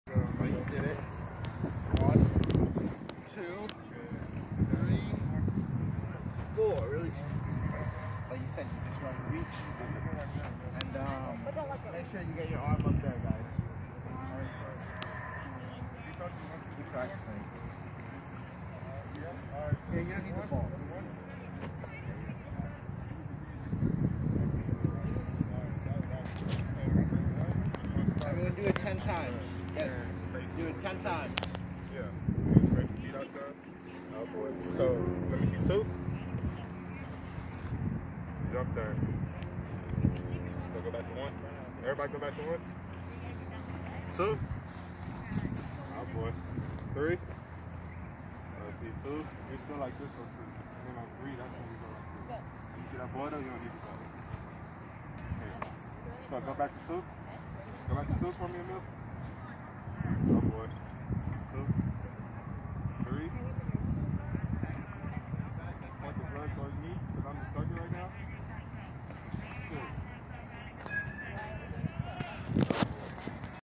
SMBA Throwing Instructional - The Towel Drill
At the SMBA weekly baseball Saturday pods, the 10U Giants learn the proper mechanics of throwing properly by starting off with the Towel Drill. November 28, 2020